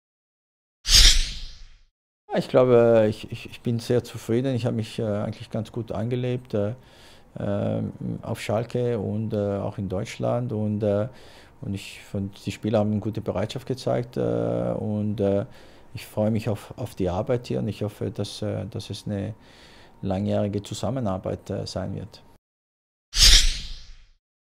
100 Tage Roberto Di Matteo auf Schalke
Der Chef-Trainer der Königsblauen feiert am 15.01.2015 ein erstes kleines Dienstjubiläum. Roberto Di Matteo ist 100 Tage im Amt.